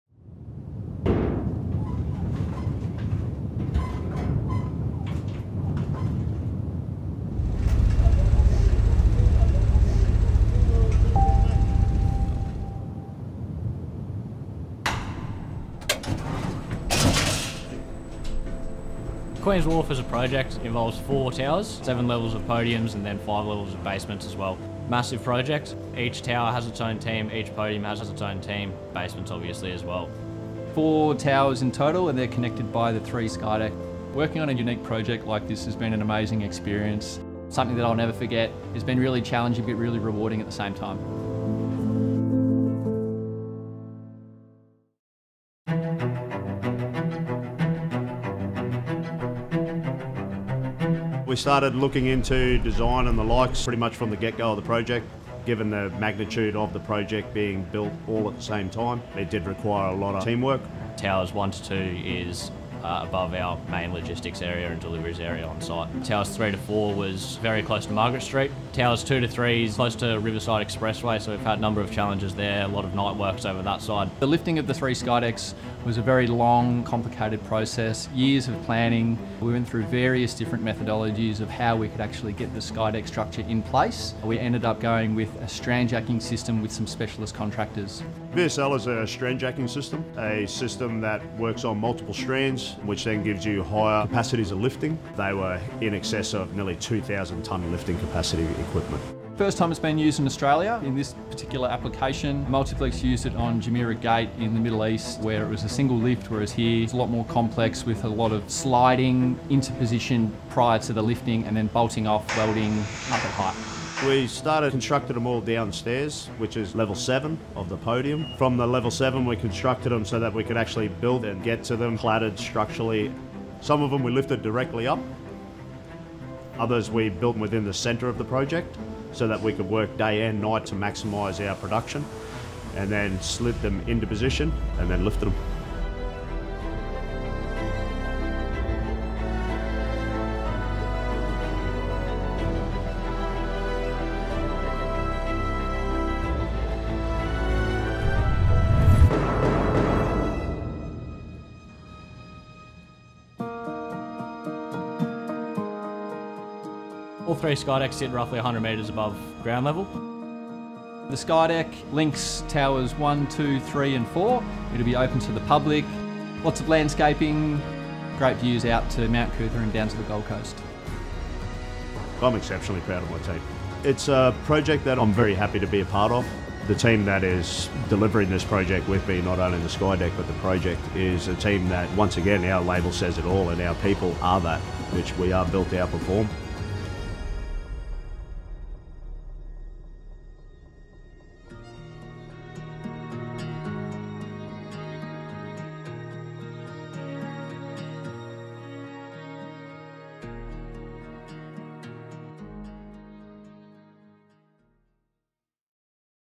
Construction of Queen’s Wharf’s Epic Sky Deck
Three years of planning. Three major lifts. One 360-degree viewing platform soaring 100 metres above the Brisbane CBD. Take a behind-the-scenes look at the complex installation of Queen’s Wharf’s epic Sky Deck and the team that helped bring it to life.